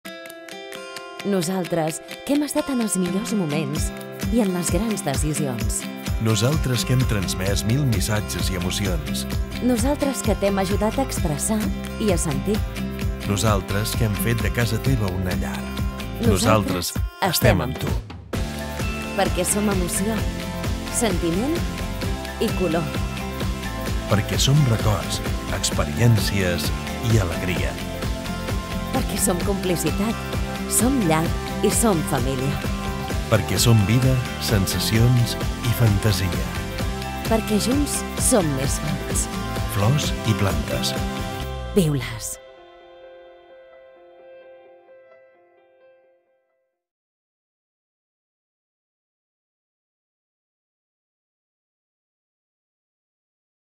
“Viu-les! Flor i Planta Viva
Presentem la campanya per a xarxes socials del sector de la Flor i la Planta Viva: “Viu-les! Flor i Planta Viva”, una iniciativa emmarcada dins del marc de l’Agenda de Comerç 20/21, el full de ruta del sector del comerç per als propers dos anys, en concret en l’àmbit de la millora de la competitivitat de les empreses.

La campanya creada pel sector i amb el suport de la Generalitat de Catalunya té com a finalitat el foment del consum de Flors i Plantes Vives, en la vida quotidiana dels catalans i catalanes.